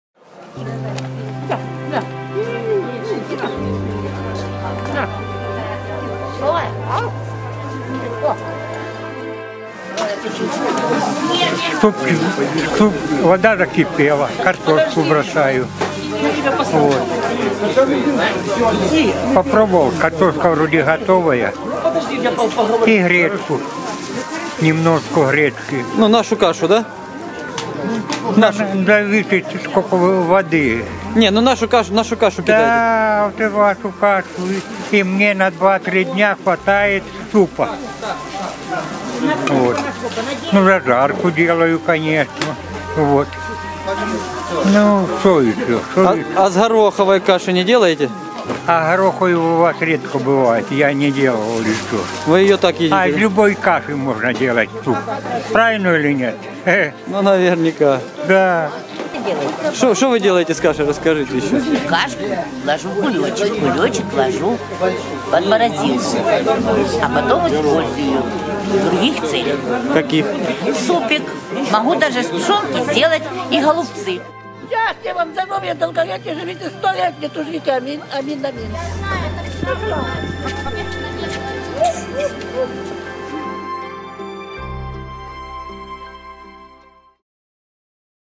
“Пища жизни Донецк“ рецепты из нашей каши
Волонтерская группа «Пища жизни. Донецк» является гуманитарной благотворительной миссией по раздаче здоровой вегетарианской пищи социально-незащищённым слоям населения, а также людям, пострадавших вследствие военного конфликта на территории Донецкого региона, вне зависимости от их расы, национальности, политических или религиозных убеждений.

Автор видеоролика - Денис Селютин